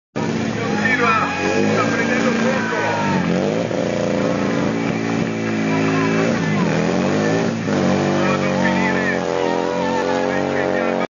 Expo Moto Pisa - stunt (parte 2)
Expo Moto 2007 di Pisa - ultimo spettacolo del 30 Aprile: uno stunt porta intenzionalmente la ruota posteriore ad infiammarsi, fino ad esplodere!
Expo-Moto of Pisa, 2007: a stunt puts on fire the rear wheel intentionally, until it explodes!